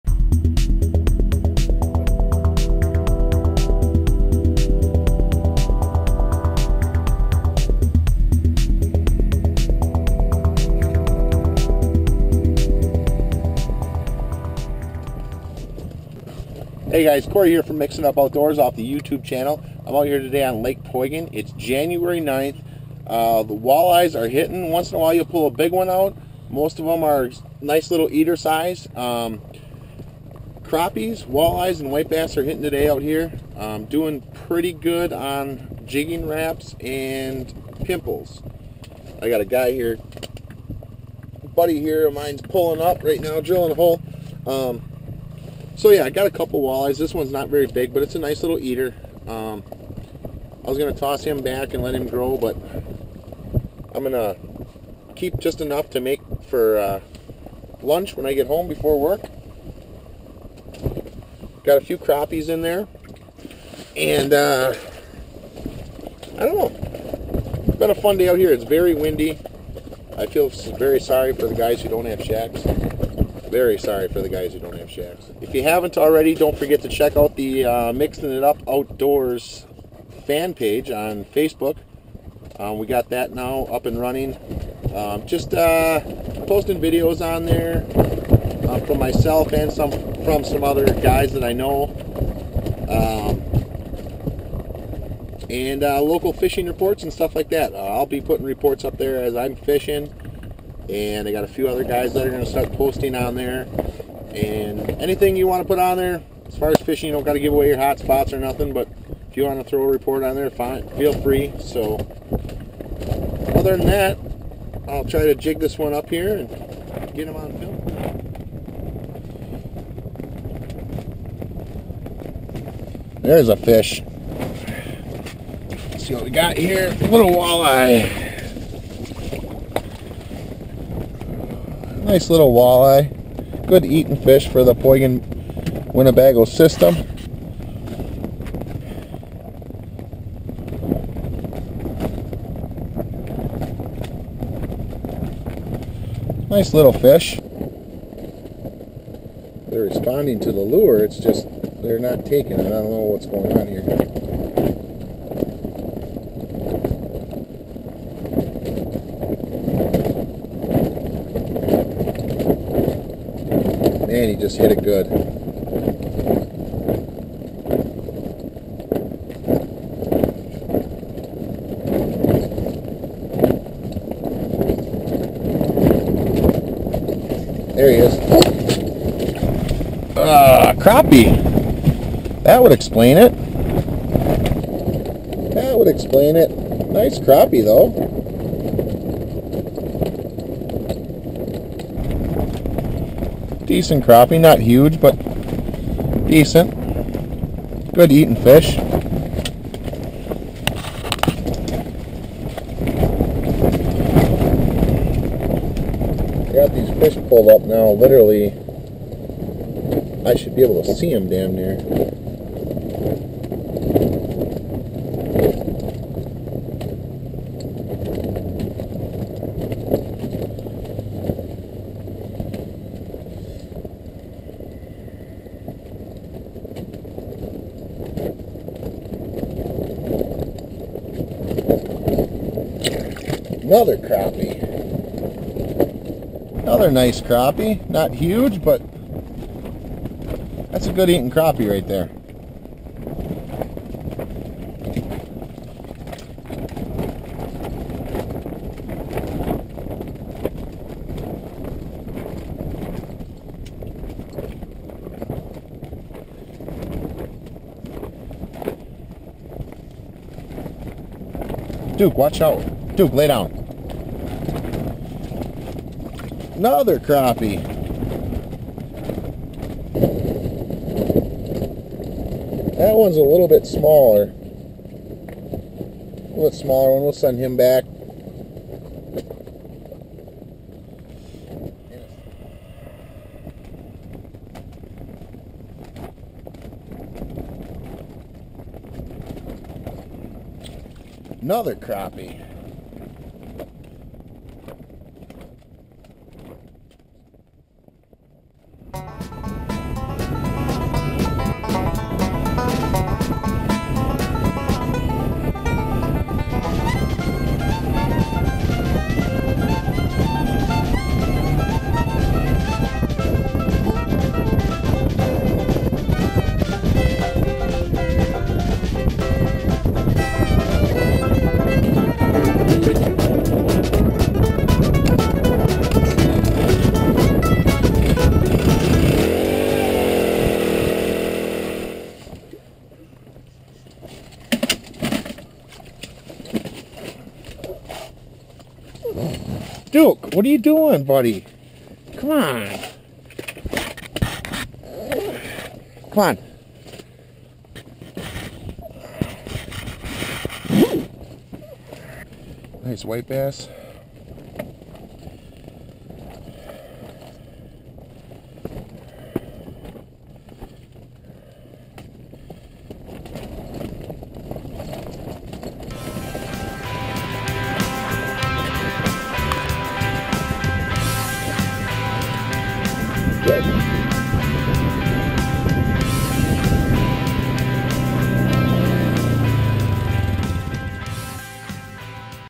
Ice fishing Lake Poygan,walleye, crappie, Mixin it up Outdoors
Kind of a slow day but still fun. Picked up a few but never really found a big school. Still a nice lunch.